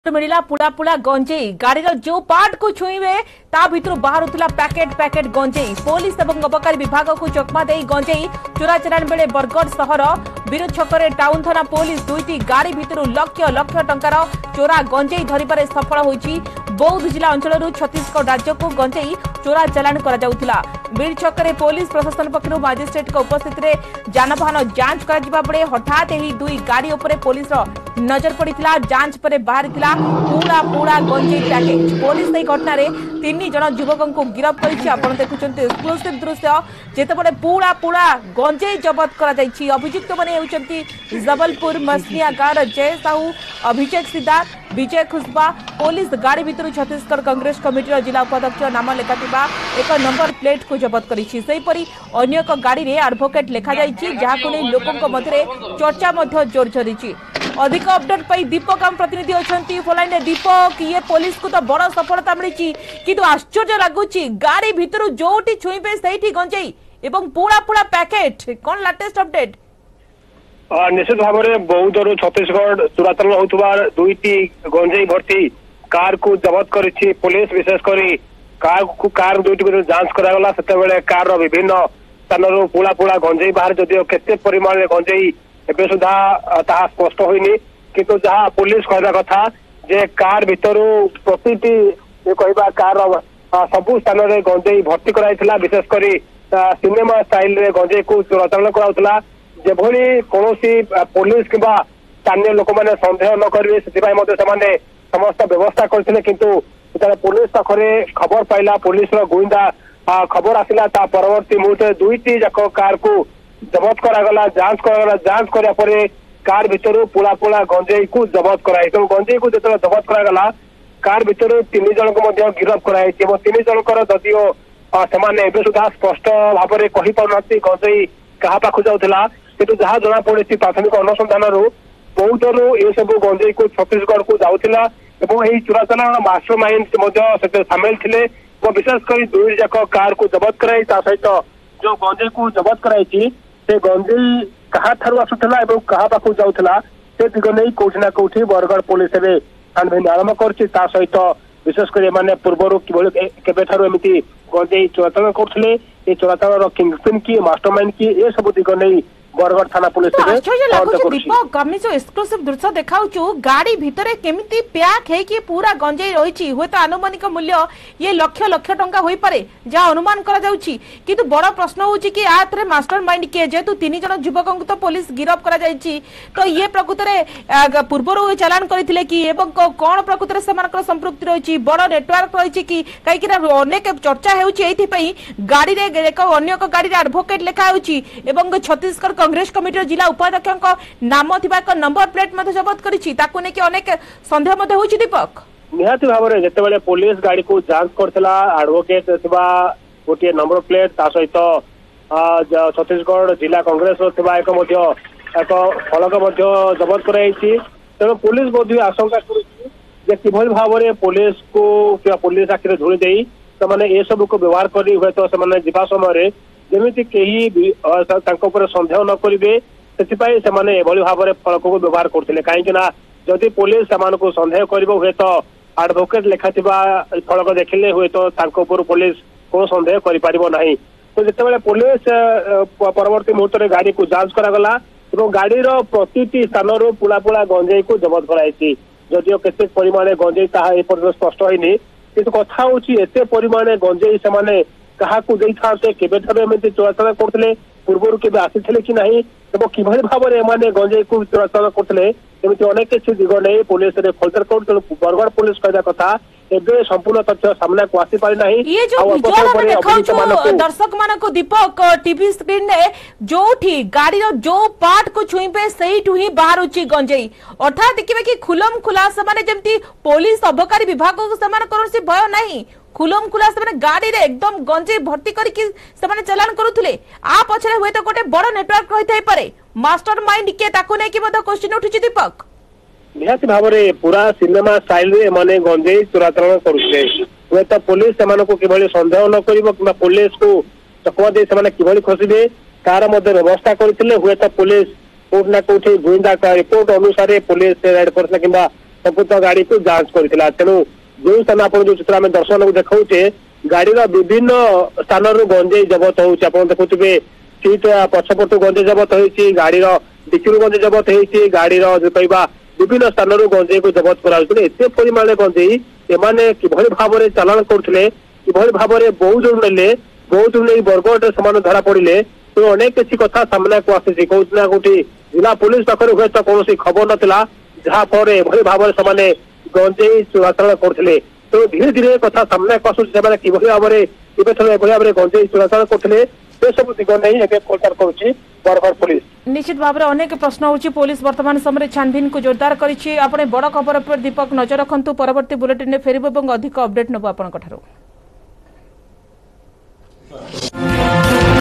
ଦୁଇଟି କାର ଭିତରୁ ମିଳିଲା ପୁଳାପୁଳା Cannabis ଜବତ | Breaking News | Odia News
Police seize cannabis more than 5 kgs, and arrested the some peoples.